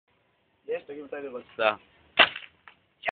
פיינטבול נגד פלאפון - cell vs paintball
ירדן שצ'ופק בעט שיעמום כבד בזמן עבודה יורה בפלאפון (שלו)

הצלם האגדי שקד רותם..